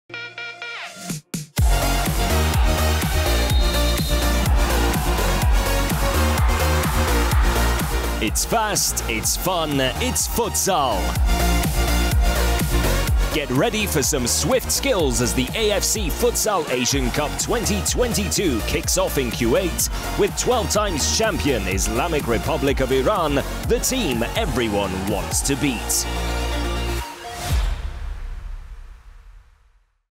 【2022五人足球亞洲盃🏃‍♂️⚽️-預告片】五人足球亞洲盃即將喺9/27開始🎉 M直播APP將會免費播出🆓
💥又快又刺激嘅2022五人足球亞洲盃⚽, 即將喺9/27開始啦🤩!
黎自16個地方嘅球隊已經蓄勢待發💪, 準備以精湛嘅傳球同接控球技術迎戰12屆冠軍得主嘅伊朗🇮🇷🏃‍♂️!
🔴M直播將會有所有賽事嘅現場直播, 並且喺M直播APP免費播出🆓!
📲記住鎖定M直播, 唔好錯過啦😎!

M直播係#2022五人足球亞洲盃 嘅澳門區獨家轉播商​
🔥亞洲頂級賽事盡在M直播, 立即下載📲 ​